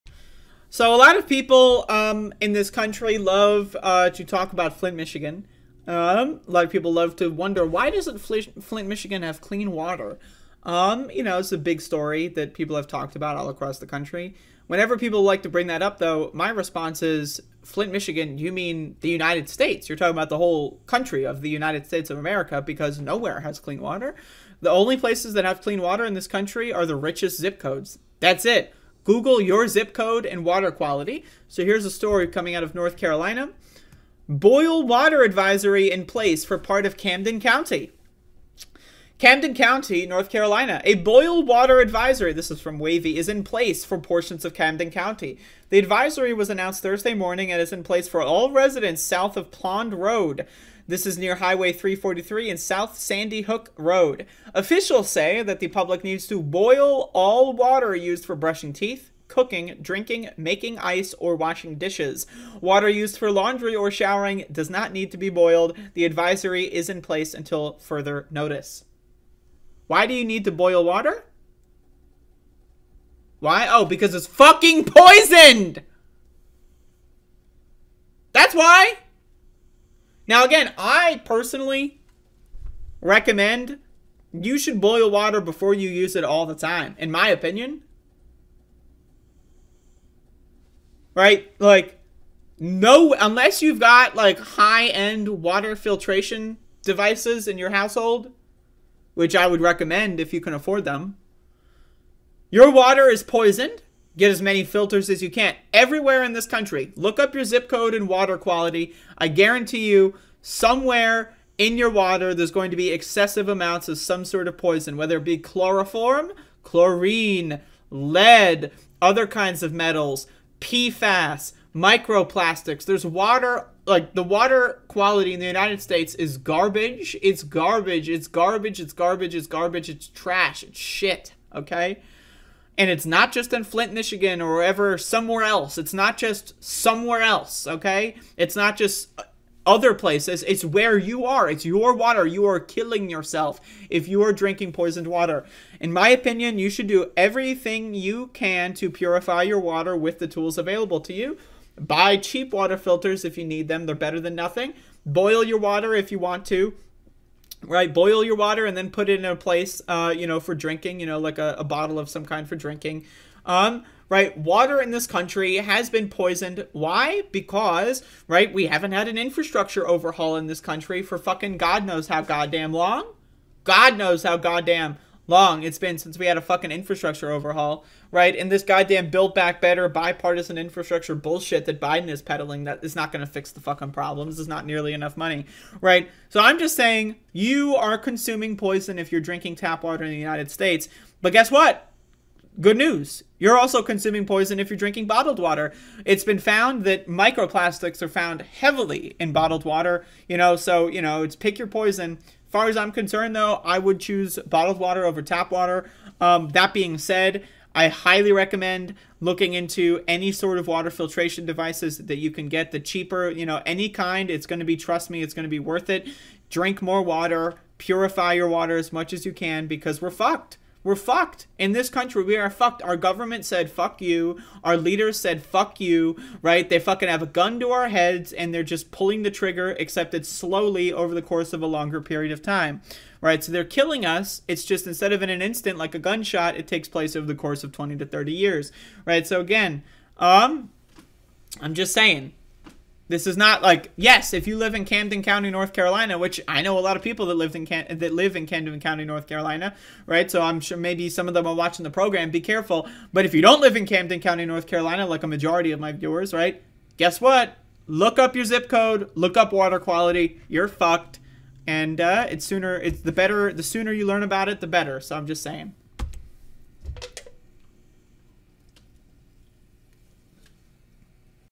Camden County North Carolina Has Boil Water Advisory Reminds Us Of Horrible Water In USA
Send me money to help me eat tomorrow!